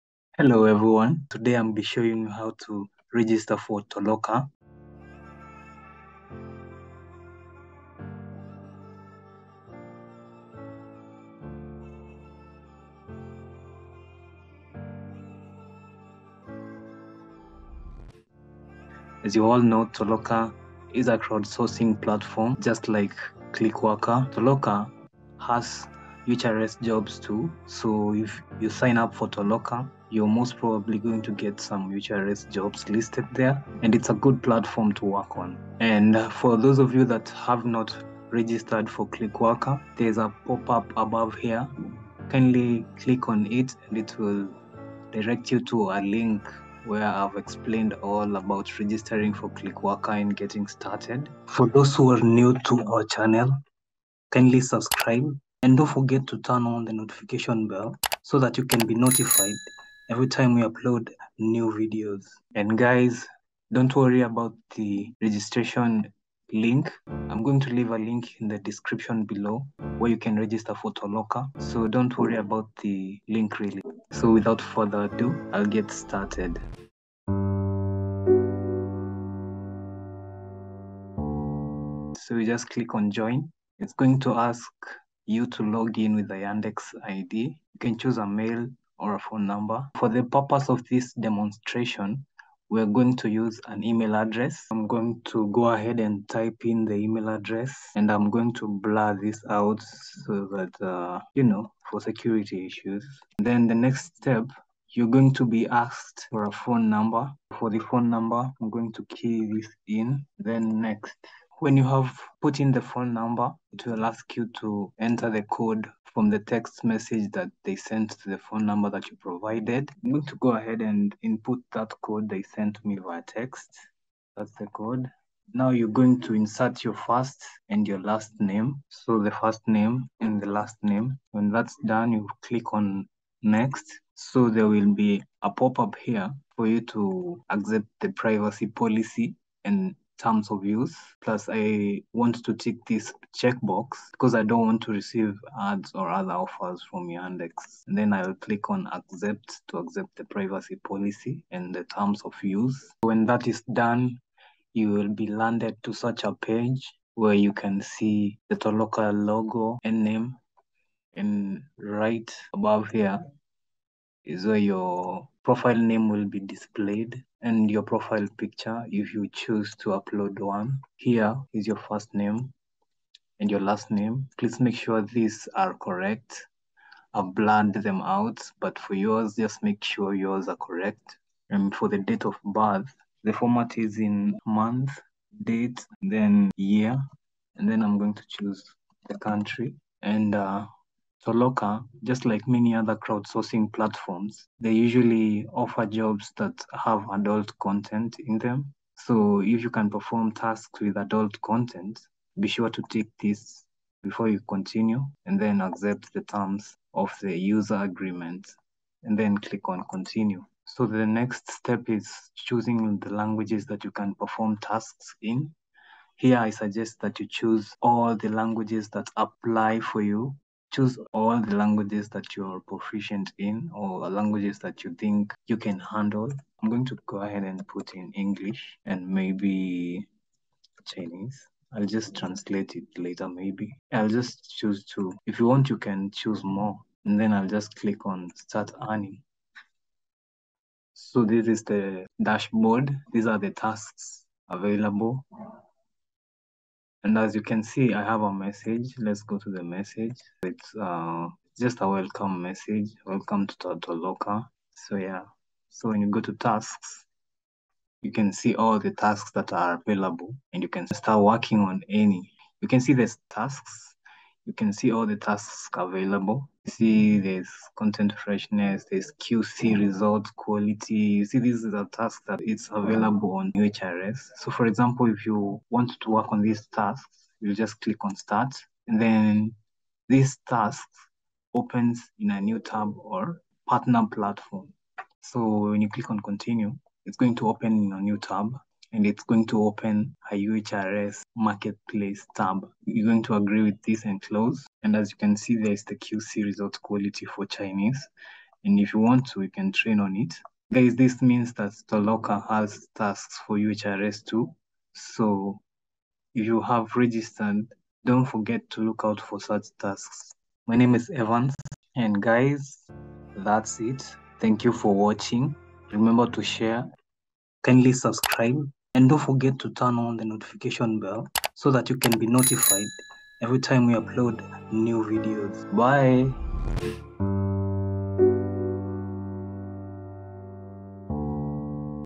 Toloka | Get Access UHRS Tasks | Complete Guide and Illustration on How To Get Toloka Account
Toloka is a crowdsourcing platform and the micro-tasking project launched by Yandex in 2014 to quickly mark up large amounts of data, which are then used for machine learning and improving search algorithms. The proposed tasks are usually very simple and do not require any special training from the performer.

Just like Clickworker, Toloka offers some UHRS tasks and the good thing about Toloka is that you can withdraw earnings anytime.